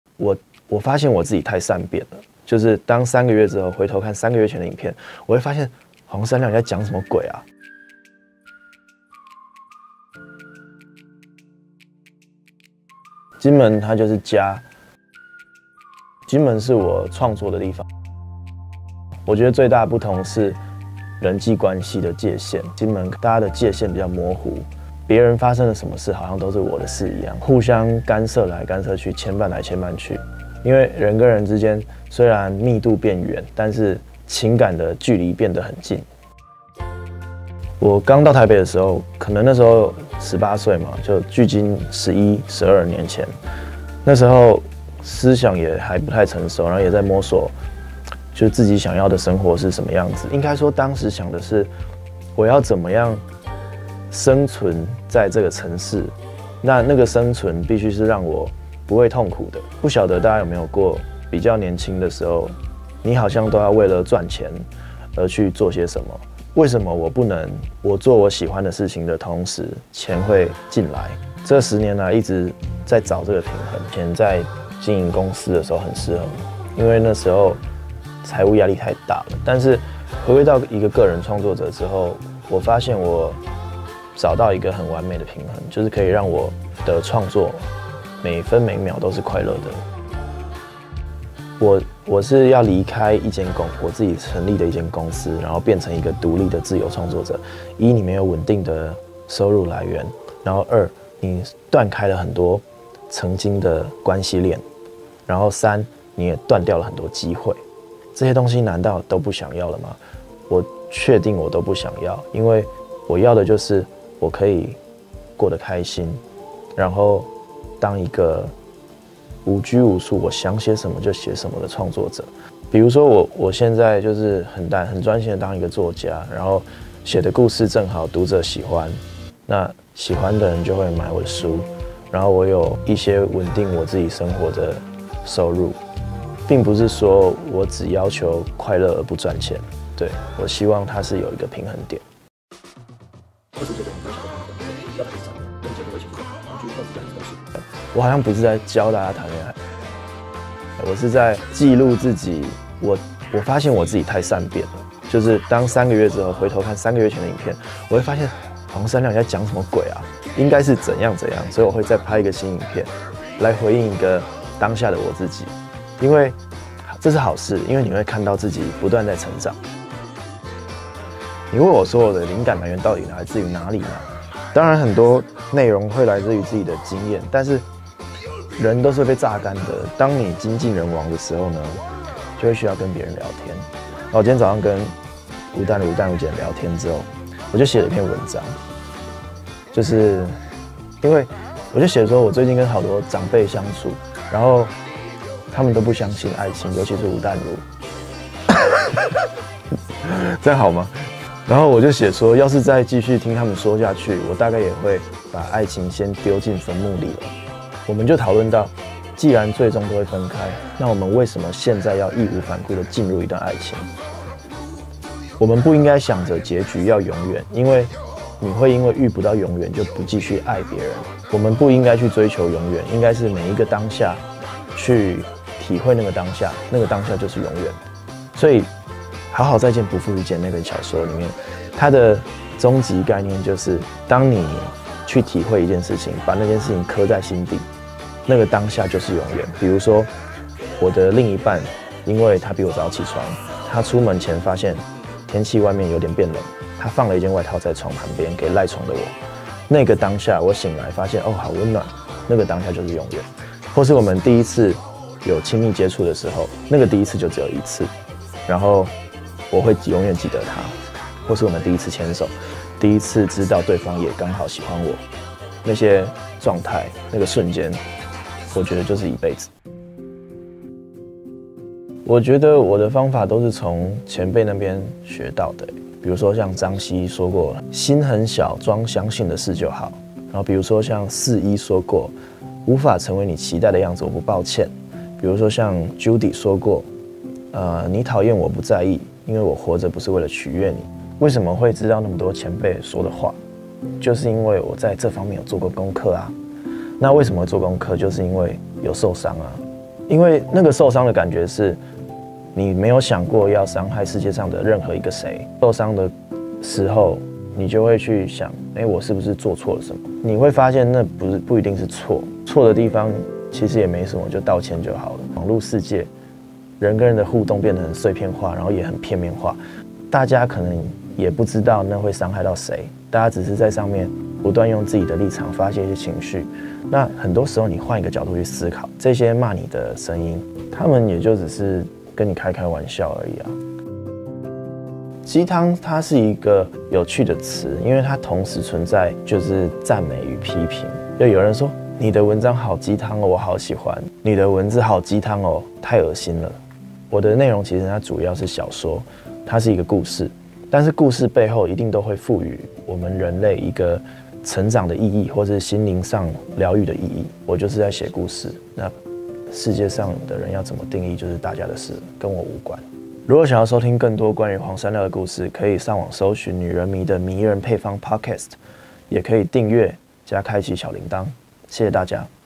【迷人配方】黃山料：面對負評，讓傷害停下來，是我照顧自己的方式
【女人迷精選專訪】

從一名服裝設計師，轉為創業家，最後放下百萬流量媒體 一件襯衫，開始專注自己喜愛的事！
29 歲的這一年，山料發現自己是一個喜歡說故事的人，哪怕使用媒介不同，但他的初衷並沒有改變。即使這些年經歷諸多負評，卻也漸漸習得轉念能力，他說，能幸運找到自己喜歡做的事情，也才能體會何謂真正快樂？也才更加喜歡自己。

透過聲音，聽更多山料的親密分享。